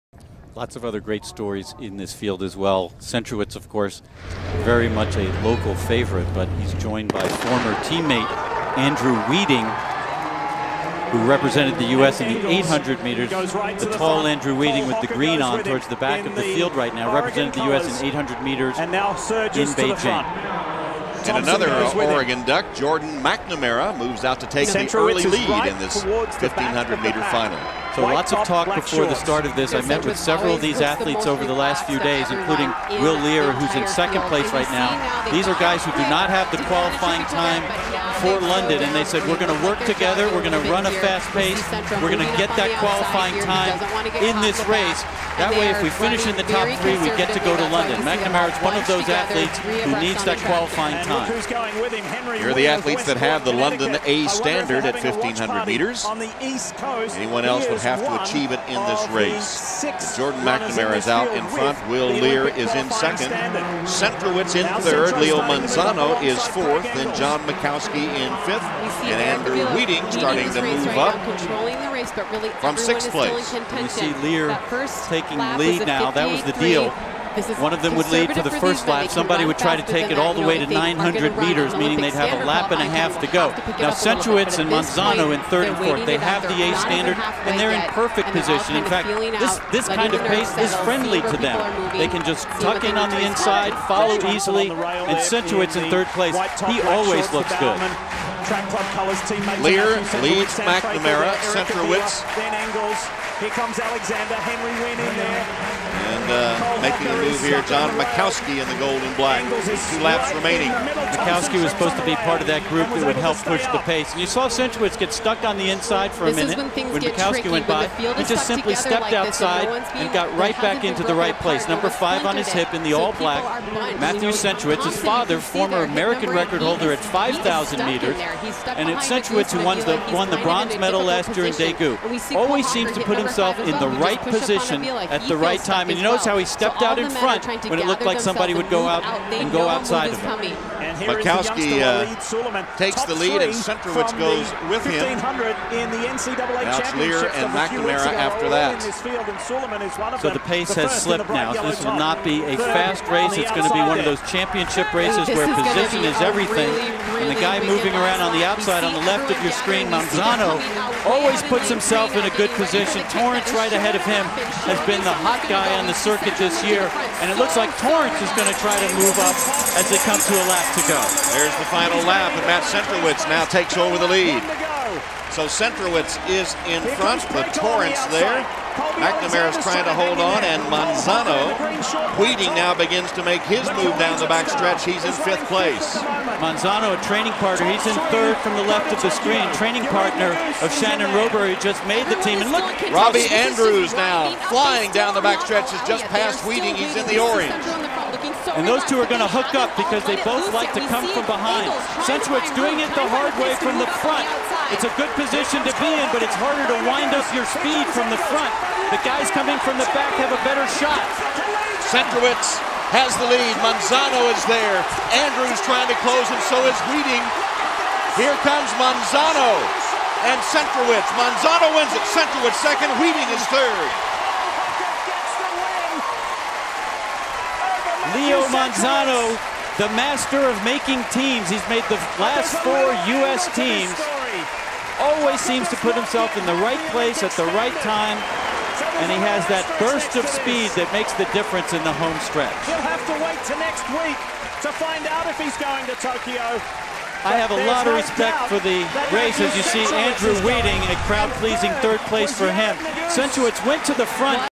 Olympic Trials Mens 1500m 2012 Next To 2021
Although Centro got beat in both of these races I think this shows why he is such an amazing runner. He knows how to control a race and how to execute his game plan.
The race from 2021 is already an instant classic. Watch Centro over the first 200m. He recognizes that everyone else is getting out faster than they need so he allows himself to fall to the back of the pack. He then uses the energy he saved from the start to 100m over the first straight away. This allows him to make the pass and get into the perfect spot.
All the respect in the world to Cole Hocker. The dude is a beast and probably has one of the more deadly kicks in the world but he is gonna need to run smarter in Tokyo if he wants to capitalize on his opportunity.